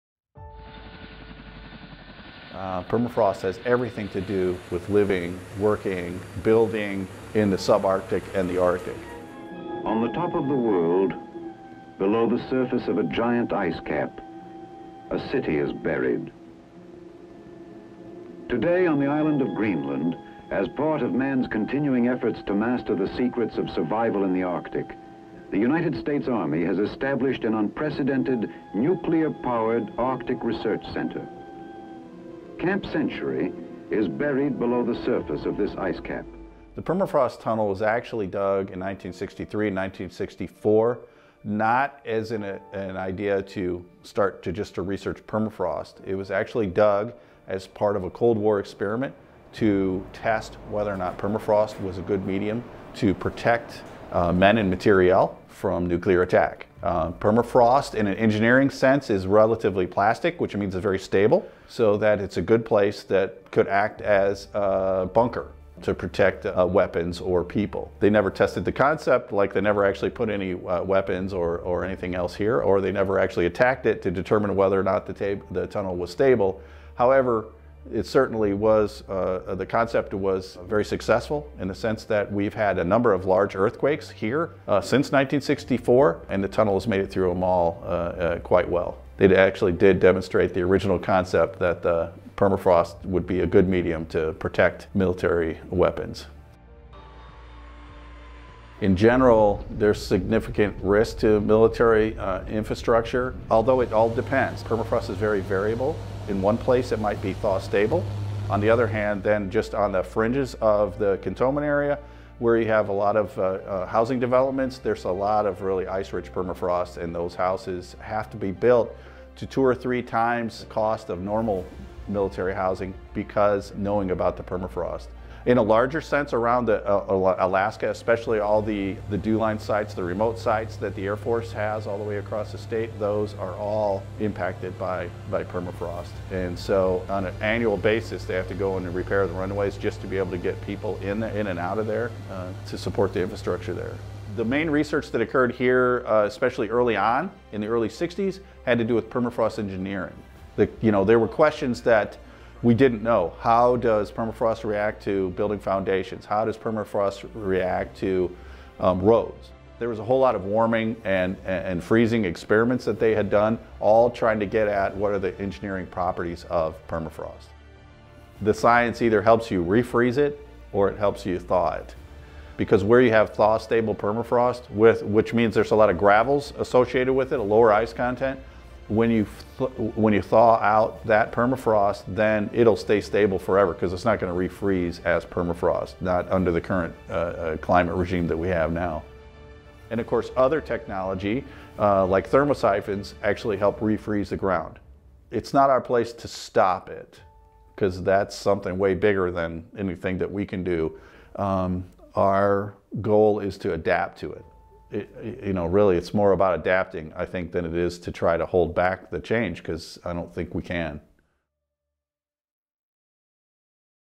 Permafrost Tunnel - Learning the Cold Truth
Nearly all of the technology in many of the world's most ubiquitous electronic devices can be traced to a single, taxpayer-funded source: the US Department of Defense. What some people don't know is a lot of engineering also can be traced back to the DoD. The Permafrost tunnels, originally created to test arctic survival has become a critical asset in developing long-lasting infrastructure in the arctic.